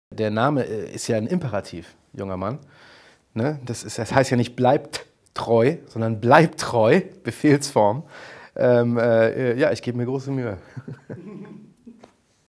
Wie hält es Moritz Bleibtreu mit seinem Nachnamen?
Moritz Bleibtreu beantwortet im Interview Fragen von SPIESSER-Lesern.